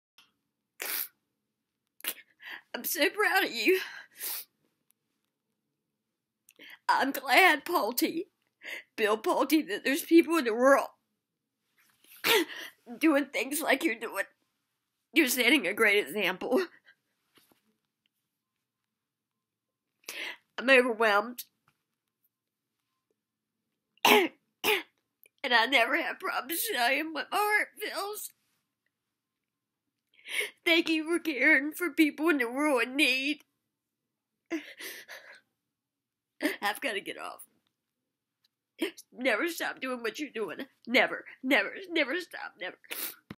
Gloria Query crying happy about Bill Pulte @Pulte Twitter Turkey drive for People in need
GLORIA Query crying happy and overwhelmed about Bill Pulte @Pulte Turkey drive for people in need